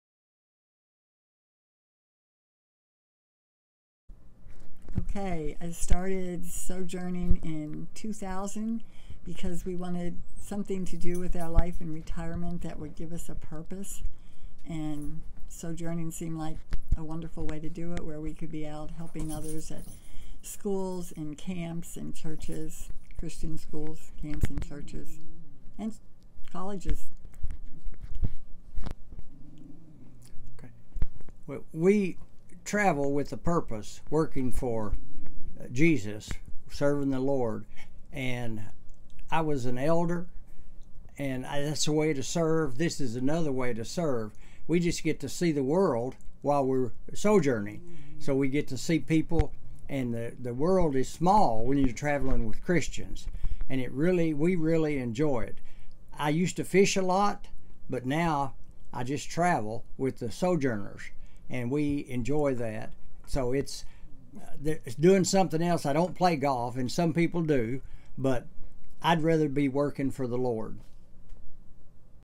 Sojourners - Lloyd & Dottie Hasty
This video is about the Sojourners, an amazing group of mostly retired senior adults who travel the U.S. in RVs, stopping at Christian campgrounds, schools, and children's homes to do whatever needs done, from manual labor to teaching and counseling. Lloyd and Dottie Hasty were among about 30 Sojourners who participated in a "sojourn" or work project at Mount Dora Christian Academy and Children's Home, in Mount Dora, Florida, USA.